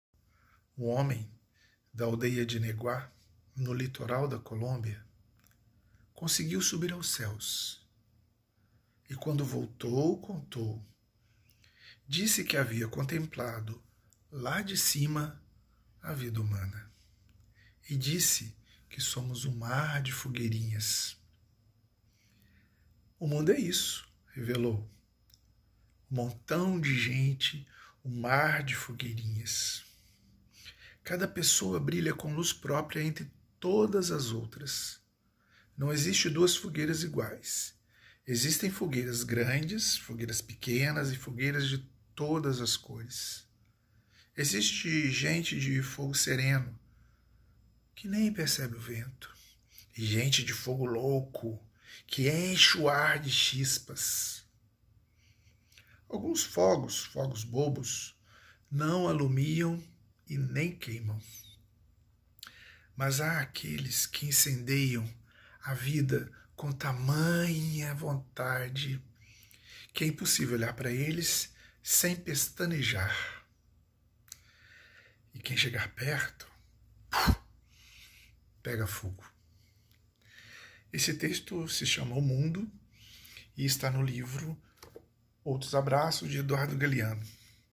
O Mundo, Eduardo Galeano
O Mundo, de Eduardo Galeano, Livro dos Abraços
Eduardo Hughes Galeano (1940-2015) foi um jornalista e escritor uruguaio. É autor de mais de 40 livros, que já foram traduzidos em diversos idiomas. Suas obras transcendem gêneros ortodoxos, combinando ficção, jornalismo, análise política e História.

Interpretação: Eduardo Santos